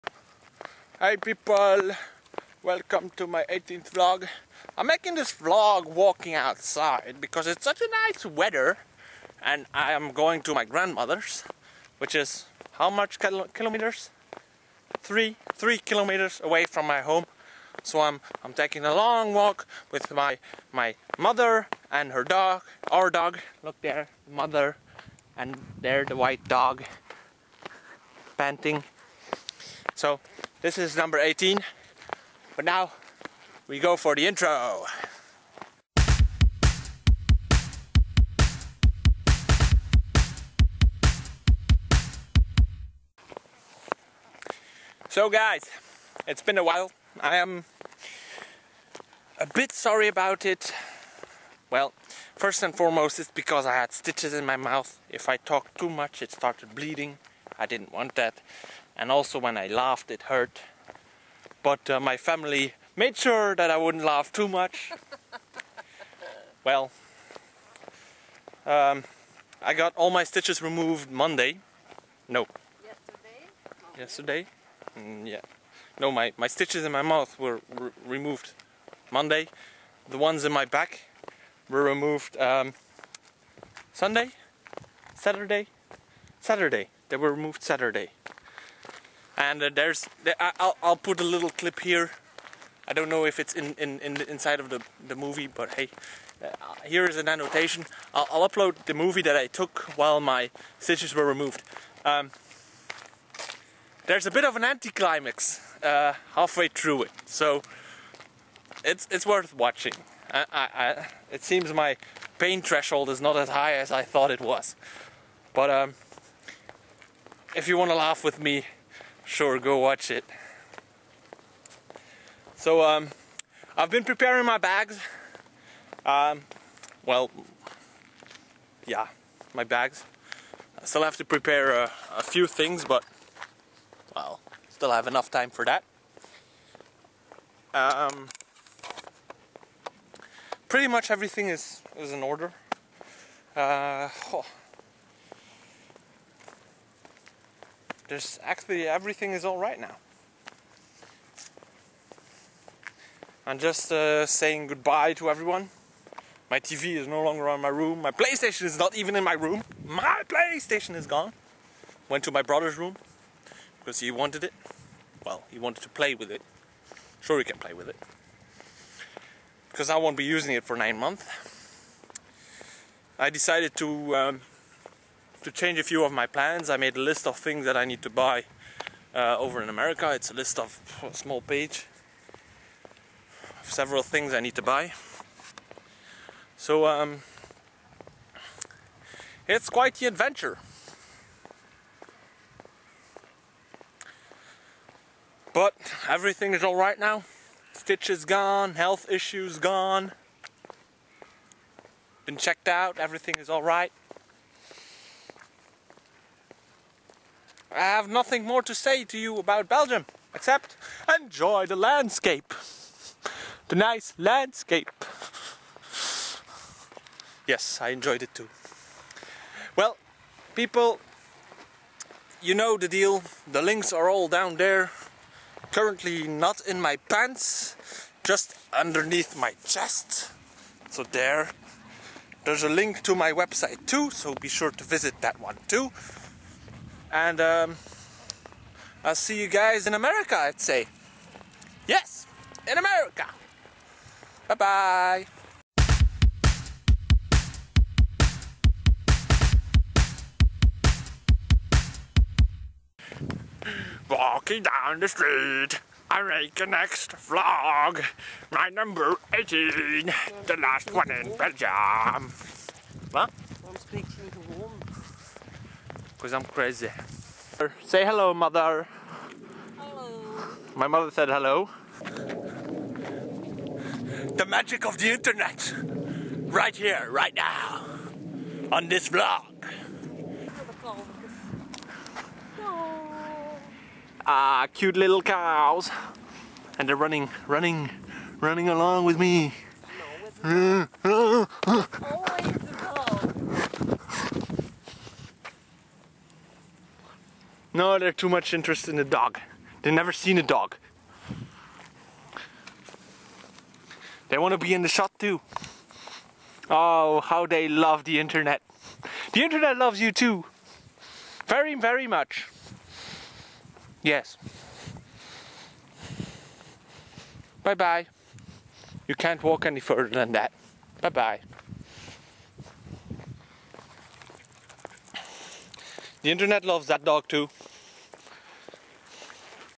IAWBA #018: The last day in Belgium
So here's my last VLOG before I'm actually in New York. Just a small heads up to you all there might be a delay between this one and the next one seeing as I don't take my laptop with me and intend to buy a new one there.